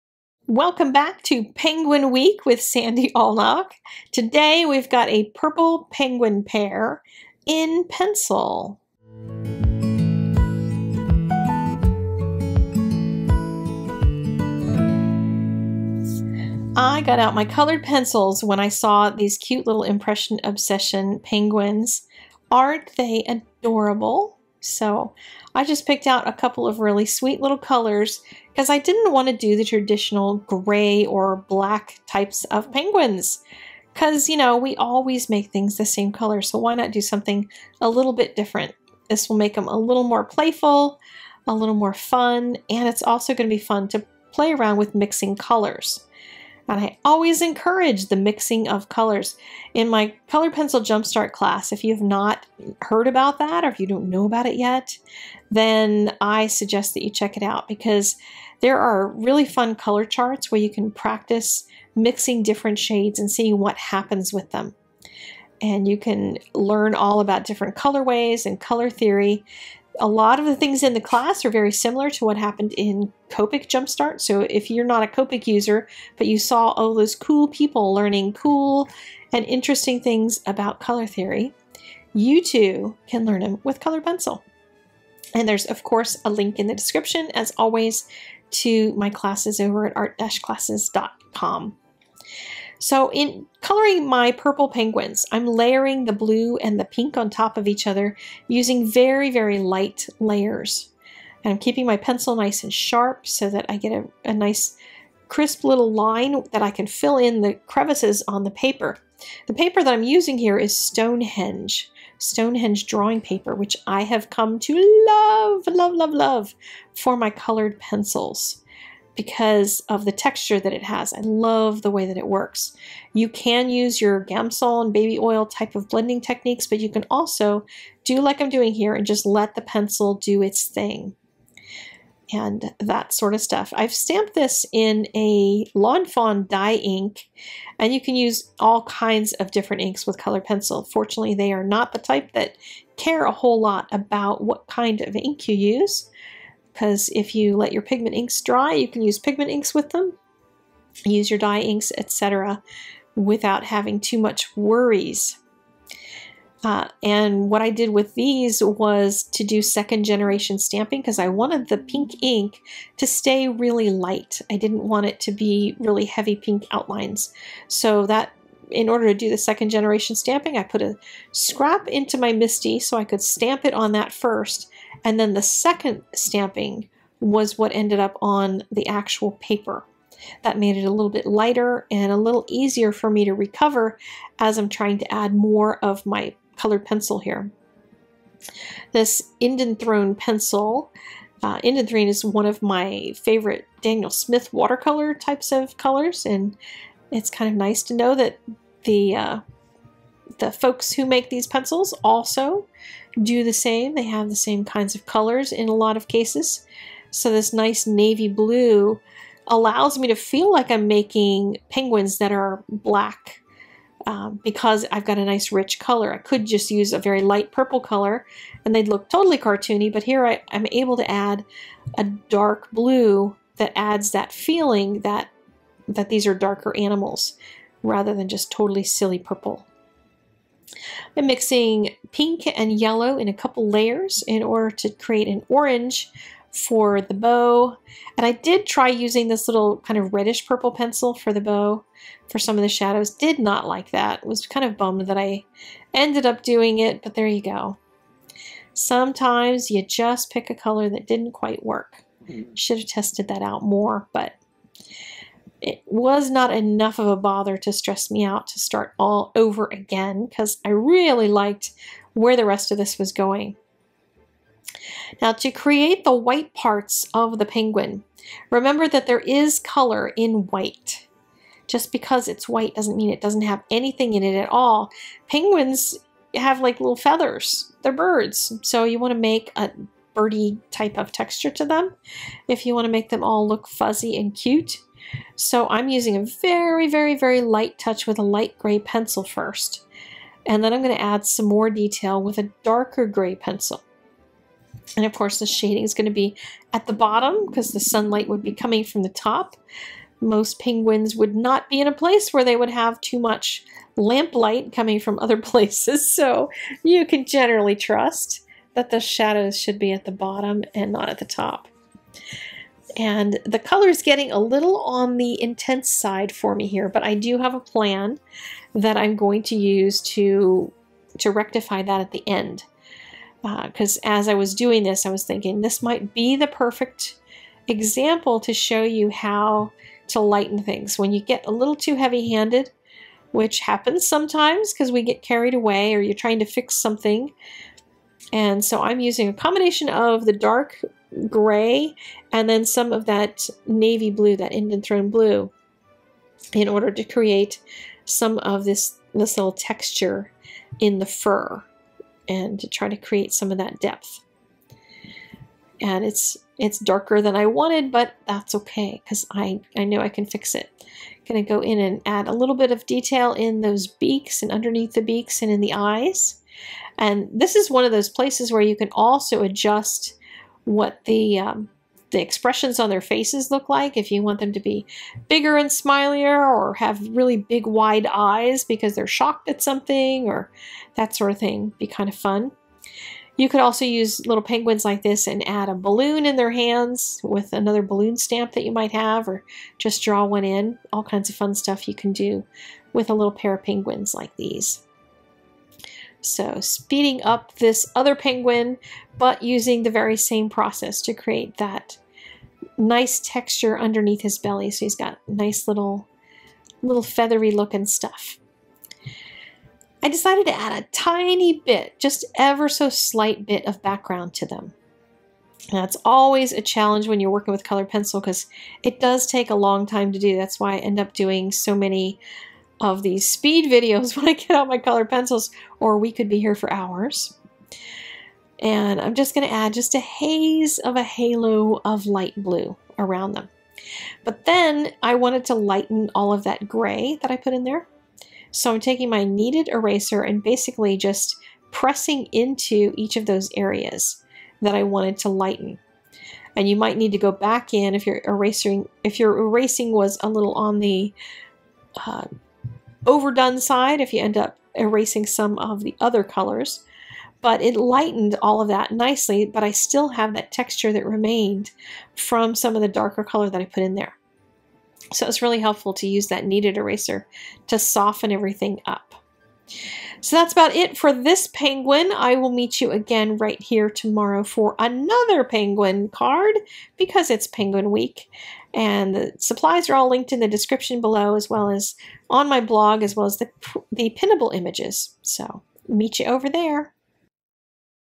Purple Penguin Pair in Prismacolor Pencil
This little pair of penguins are good for SO many seasons of cards. Of course for Christmas. But how about a loveydovey card for Valentine's Day too? Put a balloon in a flipper and it's a birthday card! Especially if you color with crazy fun colors like I did here!